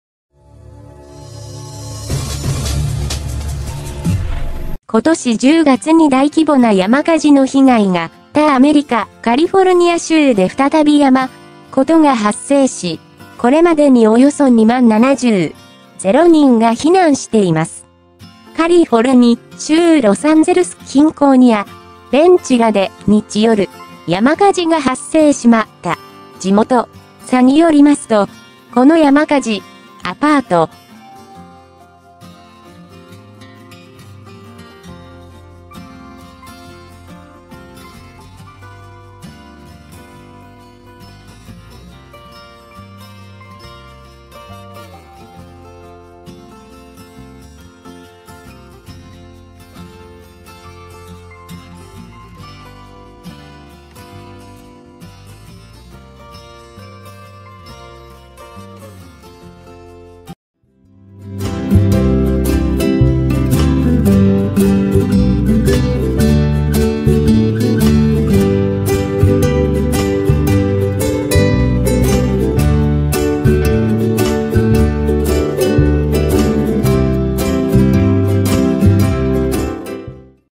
米カリフォルニア州で再び大規模な山火事
ことし１０月に大規模な山火事の被害が
たアメリカ・カリフォルニア州で再び山
事が発生し、これまでにおよそ２万７０
０人が避難しています. カリフォルニ。州ロサンゼルス近郊にあ
ベンチュラで。日夜、山火事が発生しま
た。地元。察によりますと、この山火事
アパート. ど１５０の建物が焼け、これまでにおよ
１万８０００ヘクタールが焼失したとい
ことです。また、これまでのところ住。
およそ２万７０００人が避難していま. 火は強い風にあおられて今も燃え。がっ
いて、消防当局がヘリコプターを。って
空から放水しているほか、地上で。およ
１０００人が消火活動を続けてい. すが、鎮火のめどはたっていないという
とです。山火事が発生しているベン。ュ
は、ロサンゼルス中心街から北西に。０
キロほどのところにあり、周辺には. 本人観光客にも人気のアウトレットショ
プがあります。カリフォルニア州で。こ
し１０月、北部にあるワインで有名。ナ
郡周辺で大規模な山火事が発生し、.